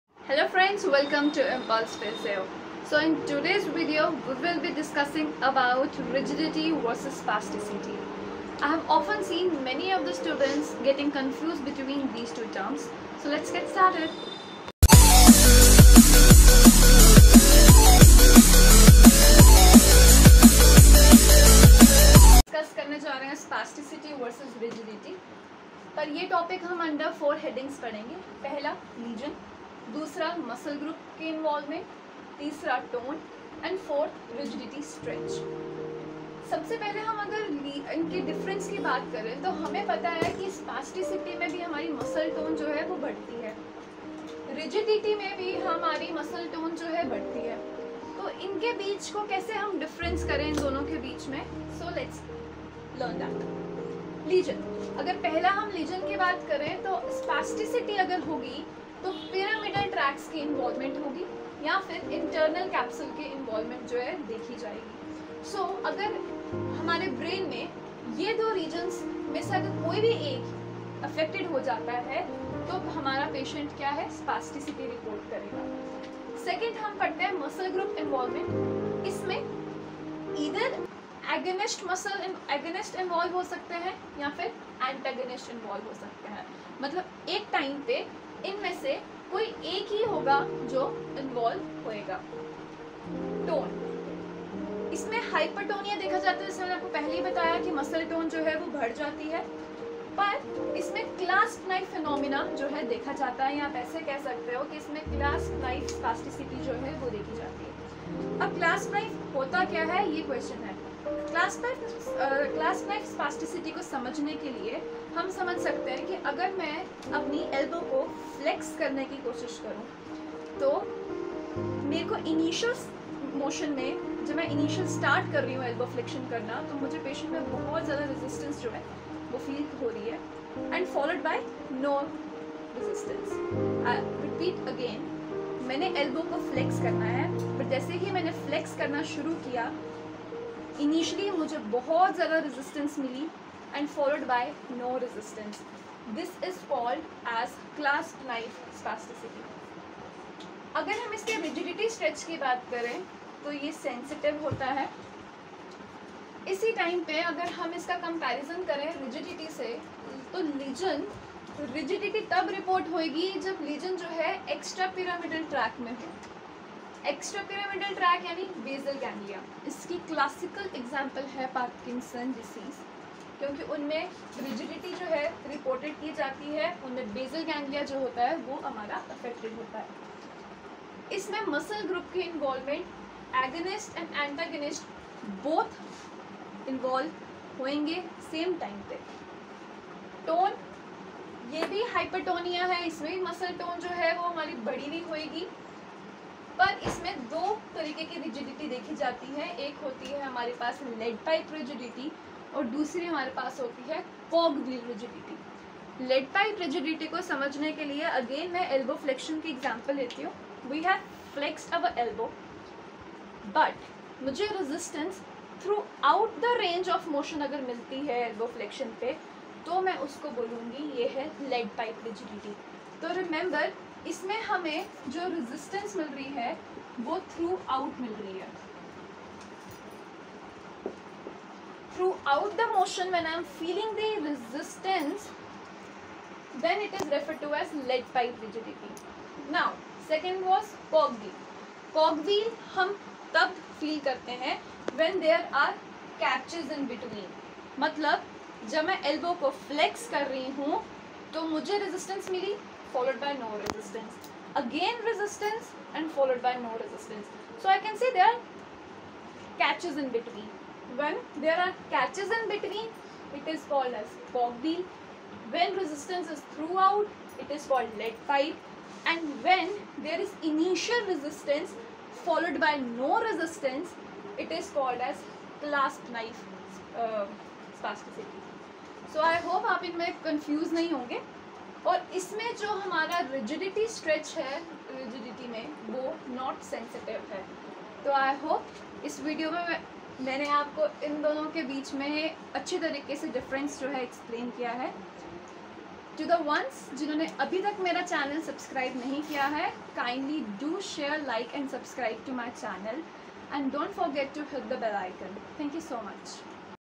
HYPERTONIA | SPASTICITY vs RIGIDITY| IMPULSE PHYSIO
Hello friends
this video explains spasticity vs RIgidity .
By the end of this video you will learn about clasp knife spasticity , cog wheel rigidity and lead pipe rigidity.